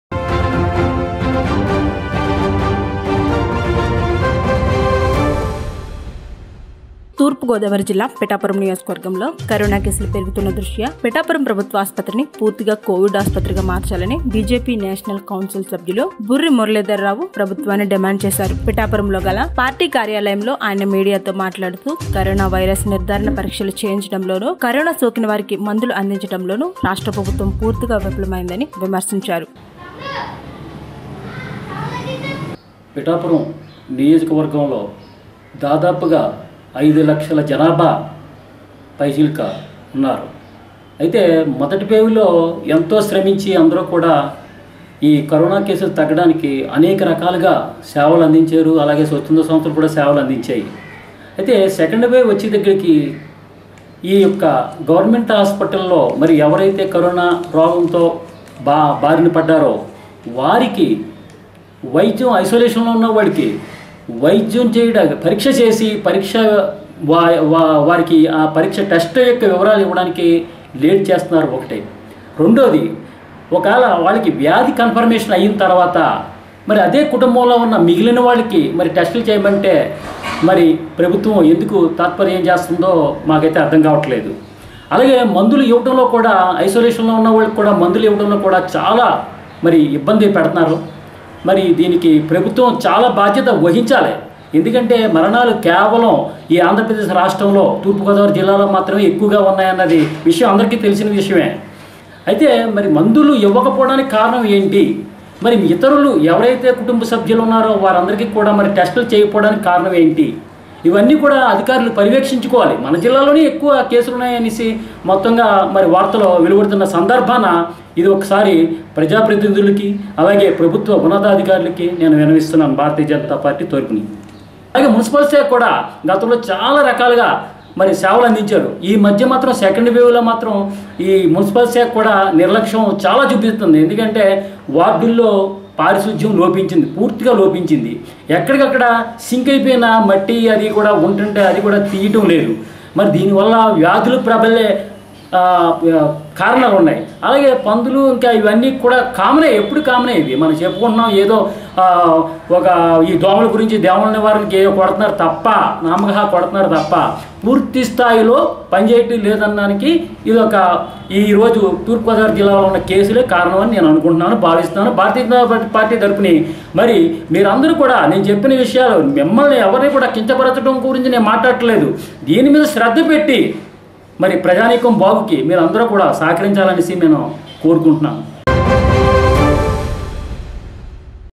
తూ.గో.జిల్లా:పిఠాపురం.కరోనా కేసులు పెరుగుతున్న దృష్ట్యా పిఠాపురం ప్రభుత్వాసుపత్రిని పూర్తిగా కోవిడ్
తూర్పుగోదావరి జిల్లా:పిఠాపురం.కరోనా కేసులు పెరుగుతున్న దృష్ట్యా పిఠాపురం ప్రభుత్వాసుపత్రిని పూర్తిగా కోవిడ్ ఆసుపత్రిగా మార్చాలని BJP నేషనల్ కౌన్సిల్ సభ్యులు బుర్రి మురళీధరరావు ప్రభుత్వాన్ని డిమాండ్ చేశారు.